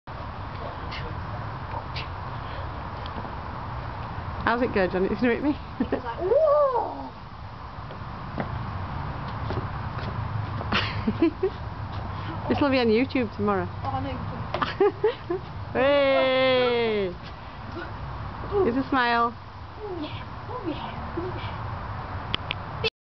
Hula Hooping Hero
The joy of hula hooping as demonstrated by a teenage boy!